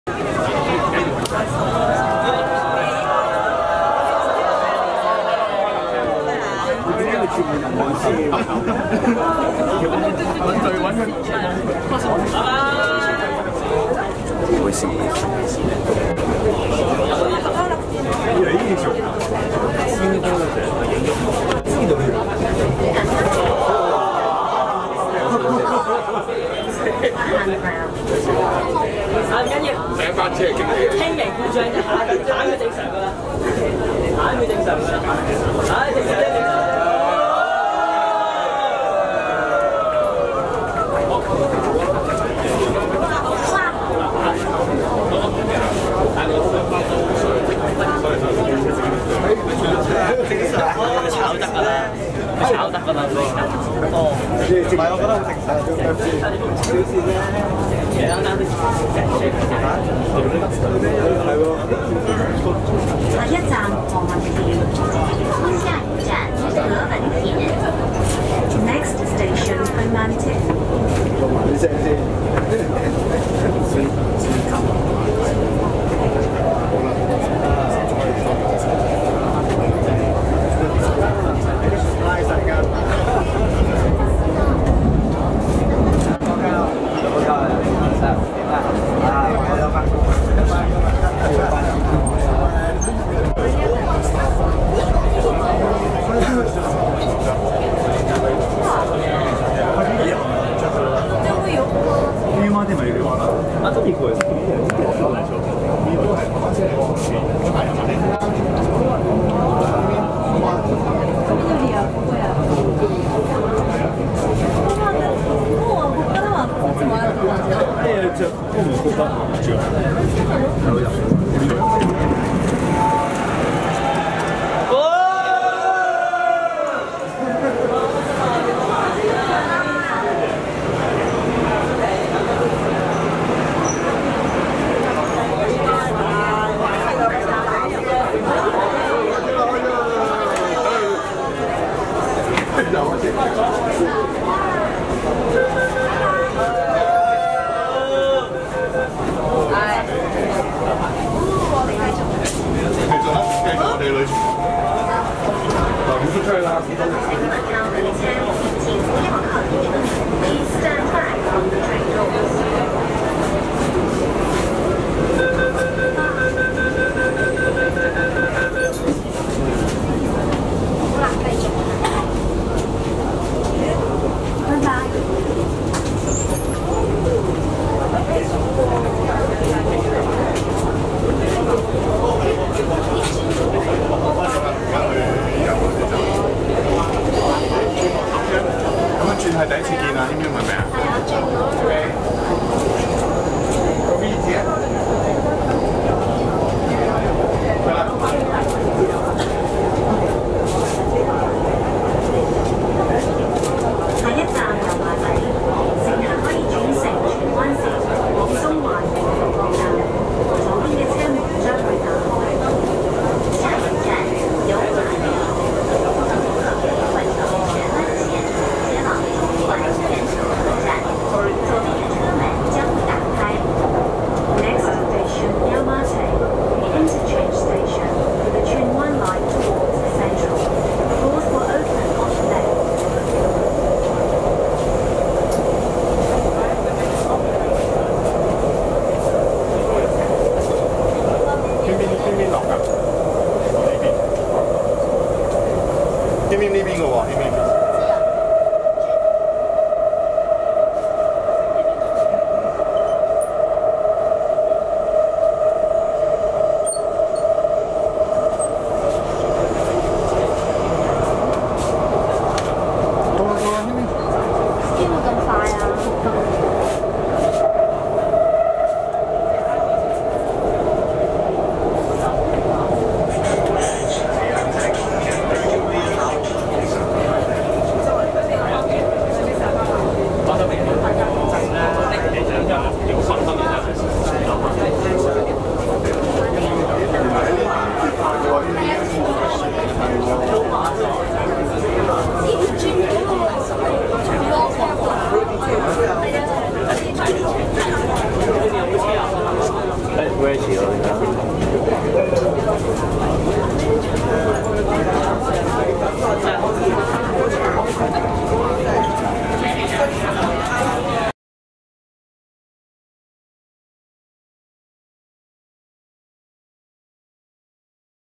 [16年10月23日]新聞第一線 - 觀塘延線首班車開出時曾突然停頓
記者：文嘉豪